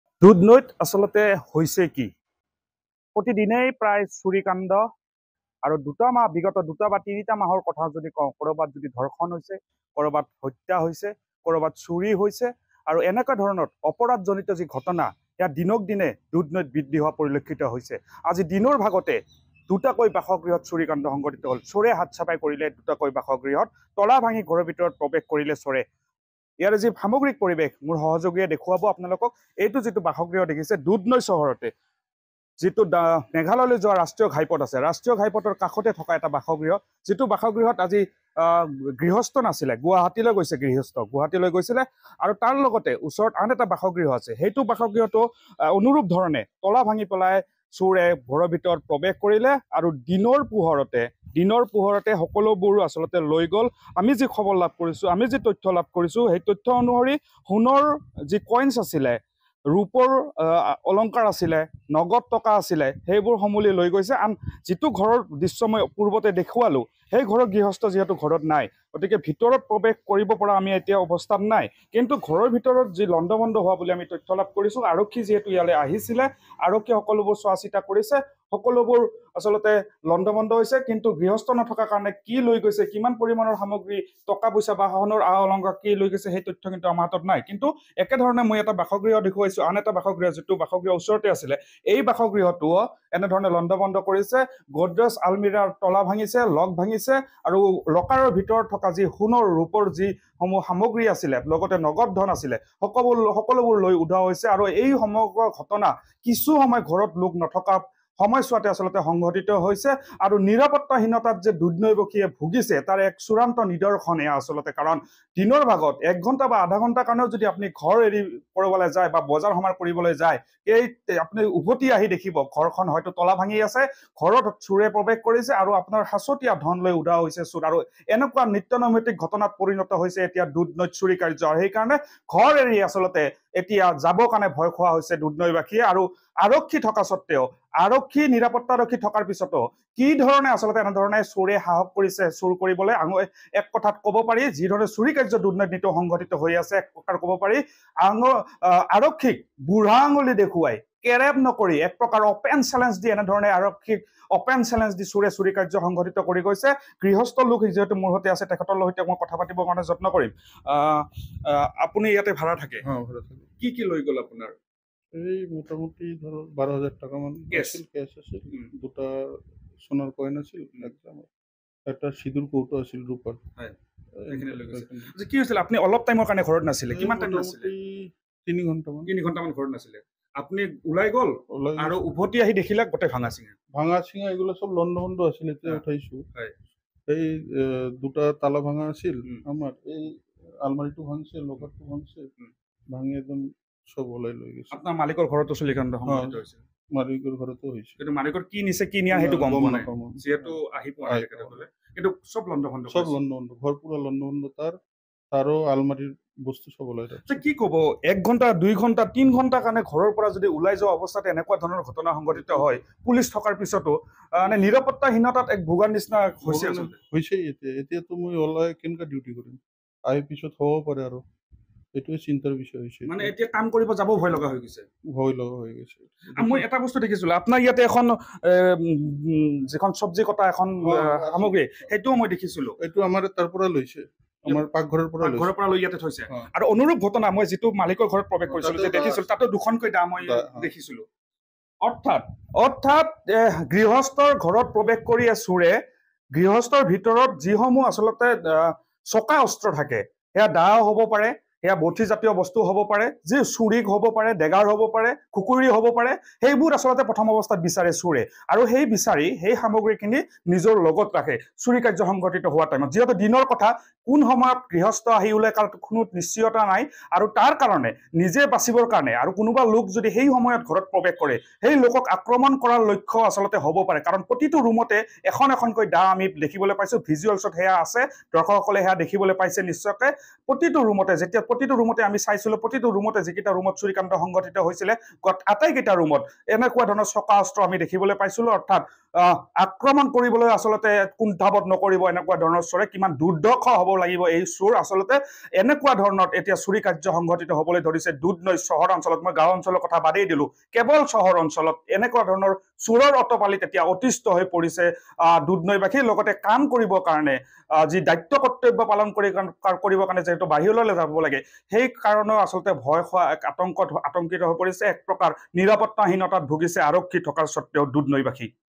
Dudhnoi News : দুধনৈত দিনৰ দুপৰতে পুনৰ চুৰিকাৰ্যৰ ঘটনা | Assam News
Dudhnoi News | দুধনৈত দিনৰ দুপৰতে পুনৰ চুৰিকাৰ্যৰ ঘটনা ৷ দুধনৈৰ দামৰা পথত থকা দুটাকৈ বাসগৃহত চোৰৰ লুটপাত। ঘৰত কোনো নথকাৰ সময়তে চোৰে লুটিলে নগদ ১২ হেজাৰ টকাৰ লগতে লক্ষাধিক টকাৰ সোণ, ৰূপৰ আ অলংকাৰ।